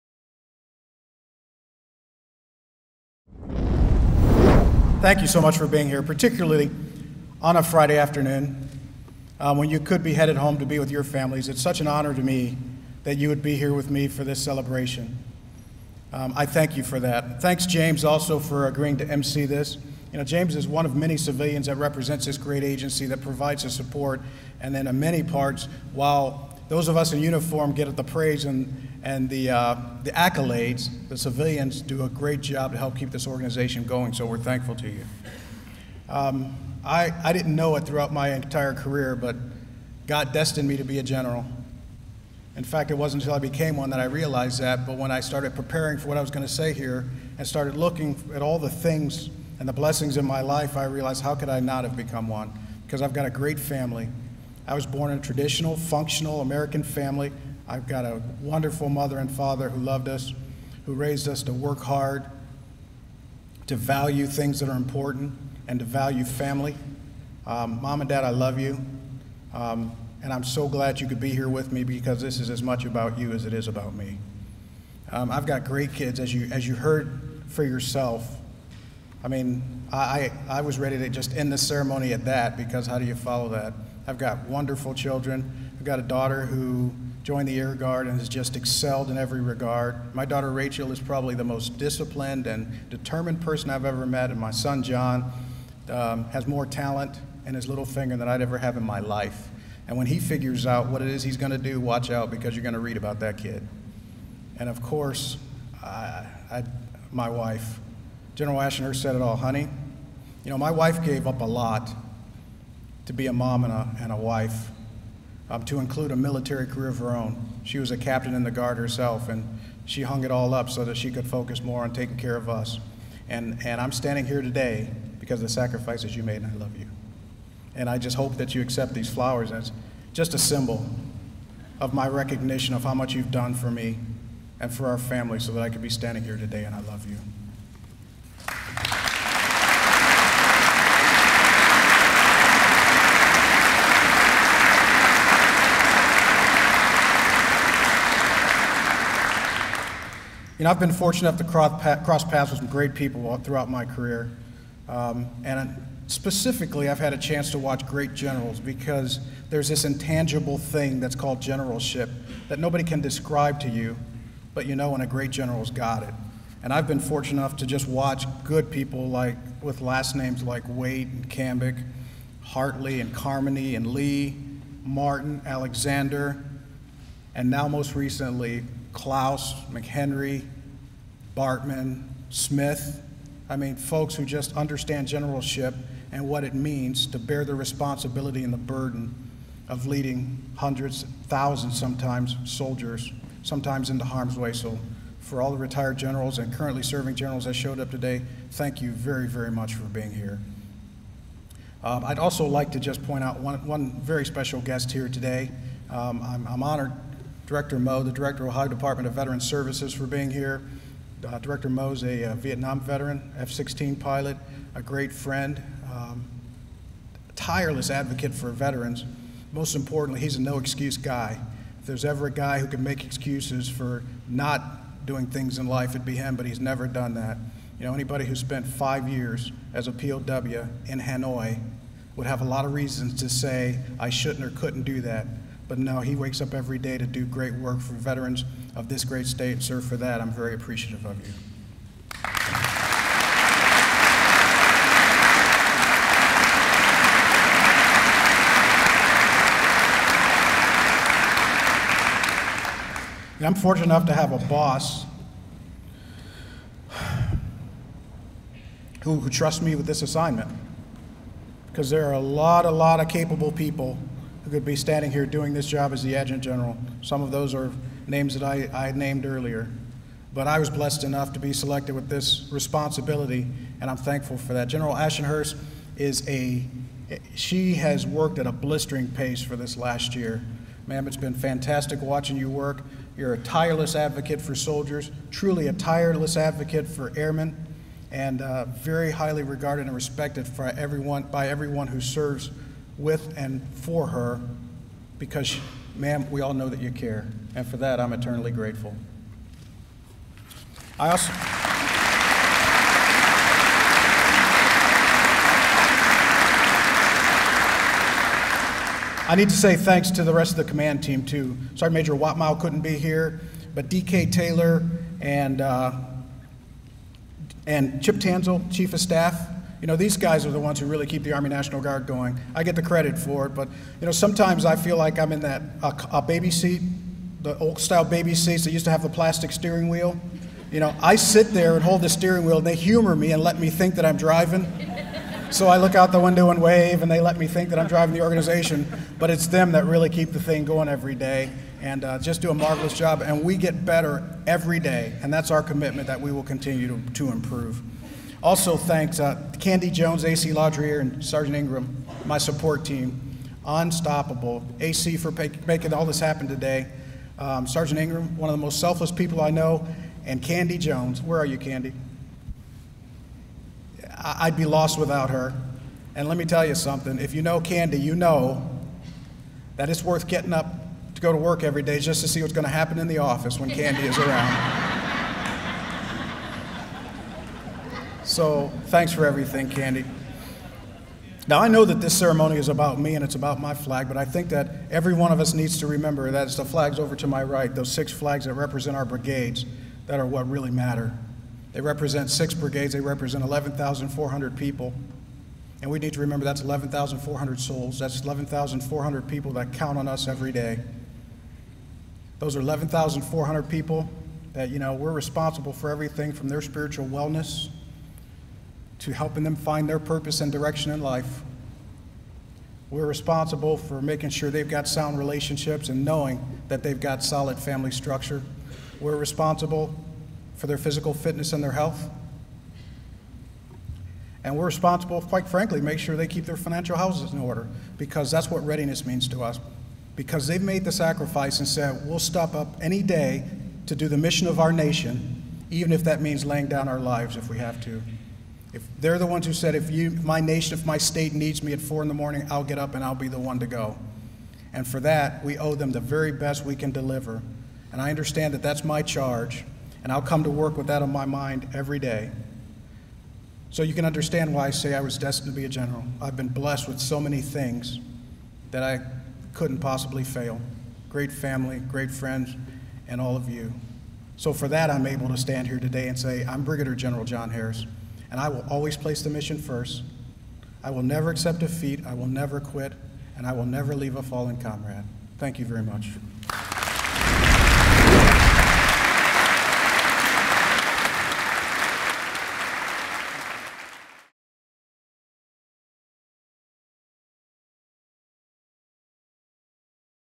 BG John C. Harris | Promotion Speech
After taking office as Ohio's Assistant Adjutant General for Army just over one year ago. Col. John C. Harris Jr. is officially promoted to the rank of Brigadier General. This clip is just after the official ceremony at the Ohio National Guard Joint Force Headquarters, located at the Maj. Gen. Robert S. Beightler Armory in northwest Columbus.